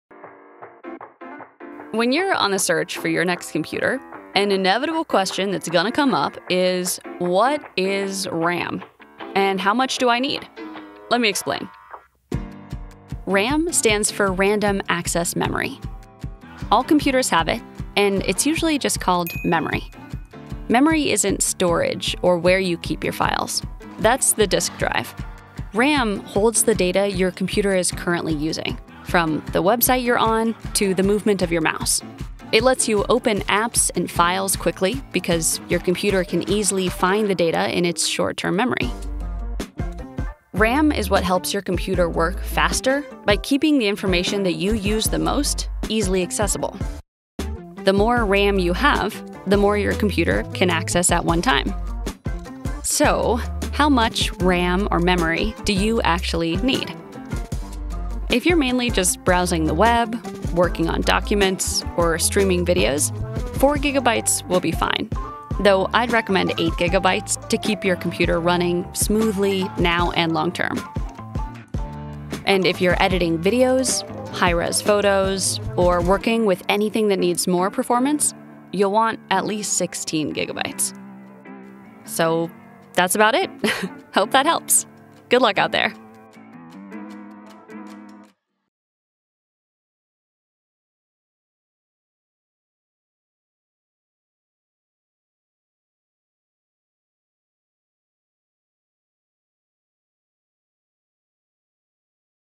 Choosing your new Windows 11 PC: How much memory (RAM) do you need?
If you’re searching for a new Windows 11 PC, you may be wondering how much RAM you need. And what is RAM anyway?

Most of us just call it memory. And unlike storage, RAM (random access memory) makes it possible to open files and apps quickly. It helps your computer run smoothly and work faster by keeping the info you use the most easily accessible. So, the more you have, the more you can access at one time. Get it?

Find out about the minimum requirements, what we recommend, and when you’ll need the maximum amount of RAM for more demanding tasks (in less than 2 minutes!).

Then you’ll be ready to choose and make the everyday easier with a Windows 11 PC that has the right amount of RAM for you.